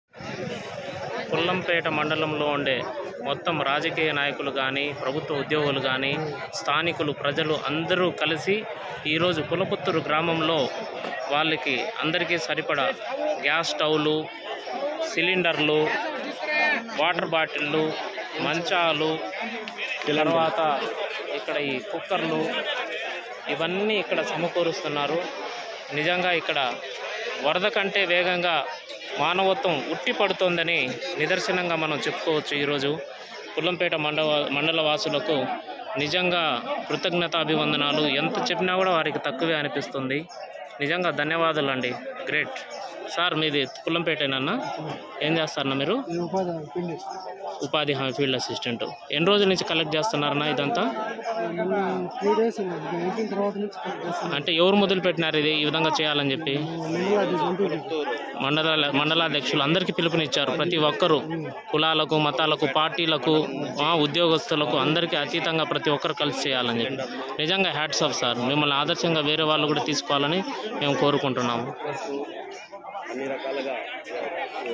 Effect || Pullapattur || Nandalur Floods news || kadapa cyclone
Heavy rains in Kadapa, resulted in floods and washed out villages near nandalur